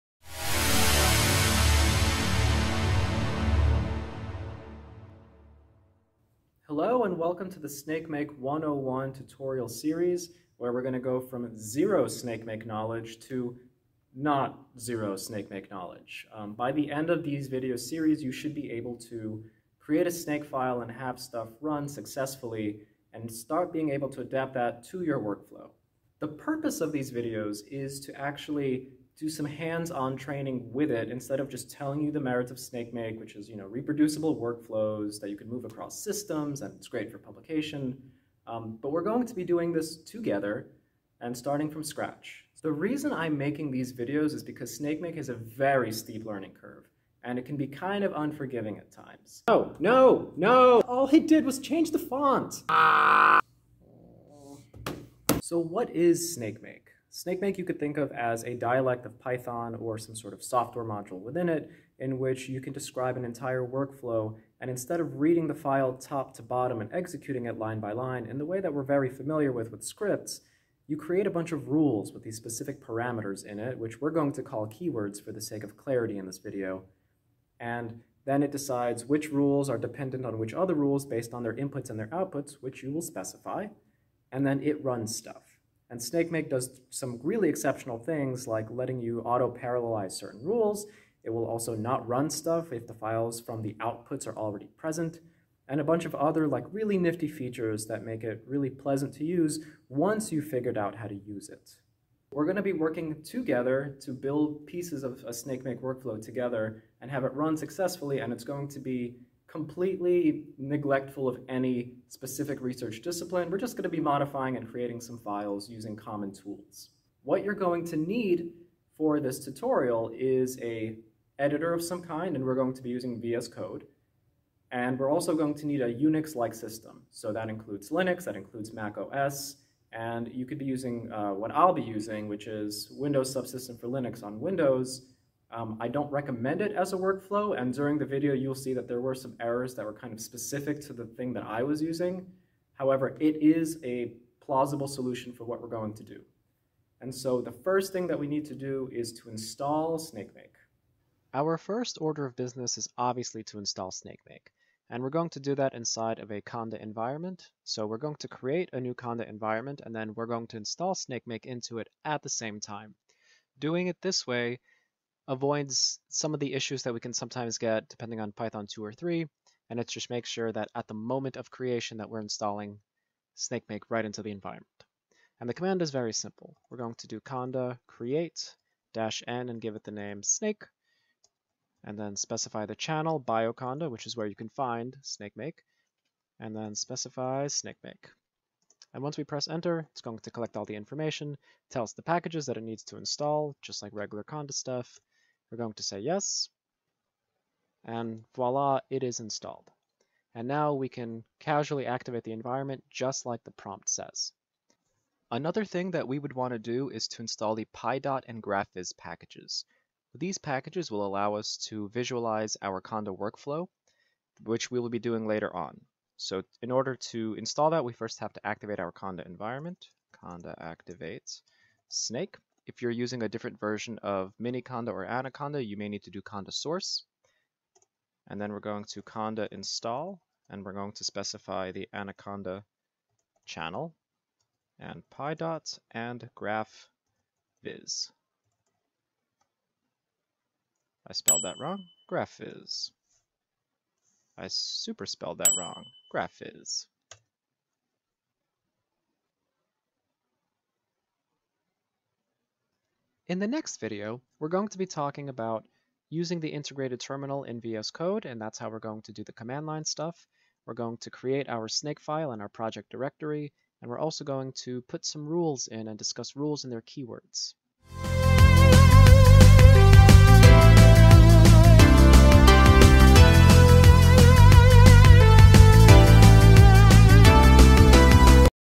Snakemake101 pt.1: What is snakemake and how do we install it?
We discuss what snakemake is, why you might want to use it, and install snakemake together.

GitHub: pdimens